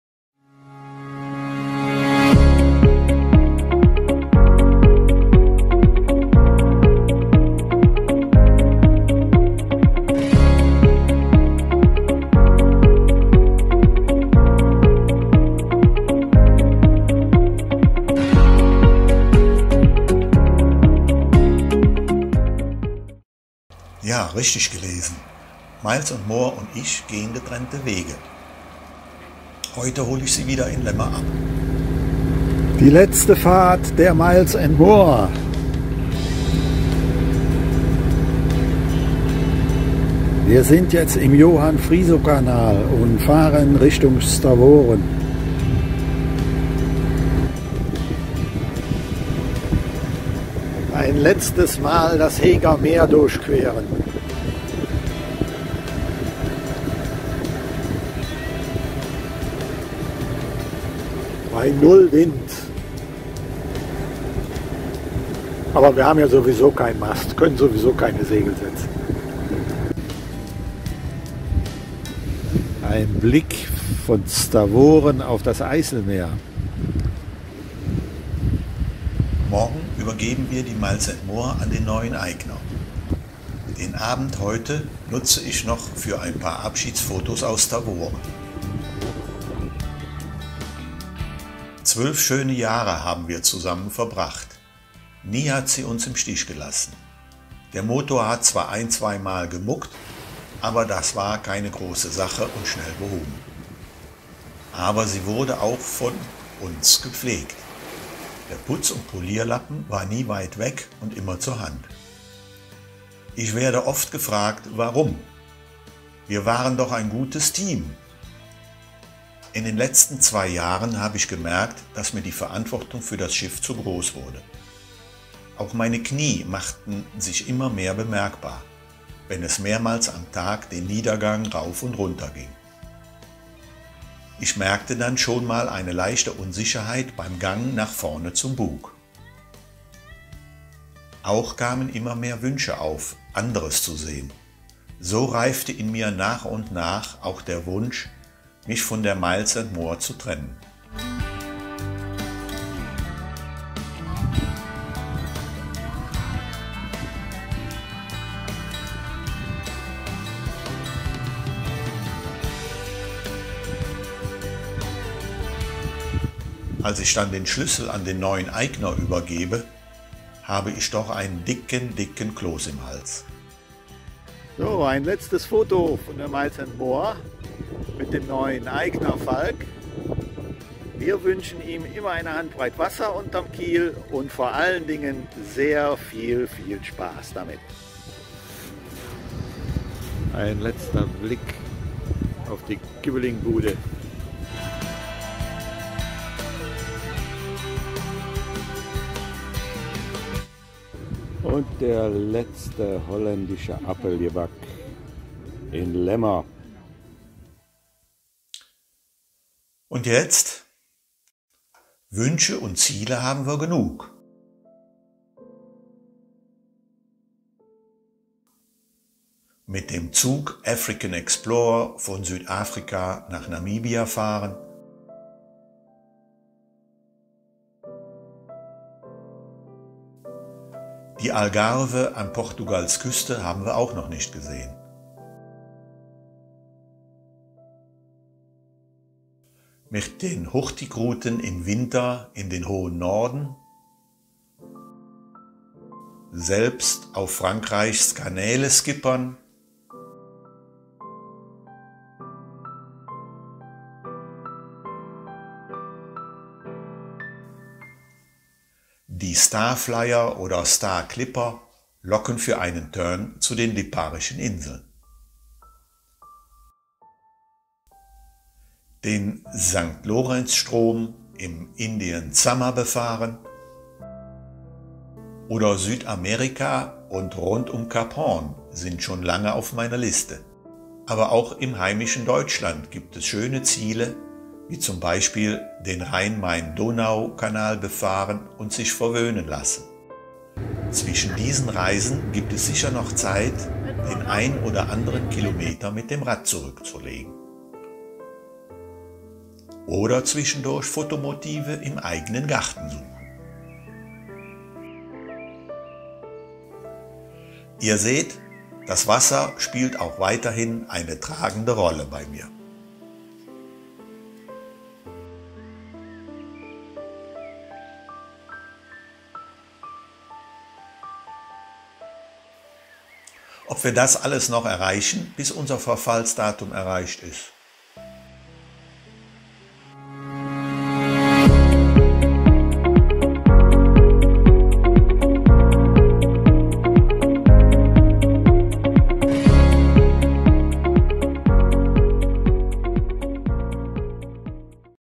Getrennt von Tisch und Koje - Wat nu.....?
Abschied von der MILES & MORE und ein paar letzte Bilder von unserer zweiten Heimat.
Wie geht's weiter?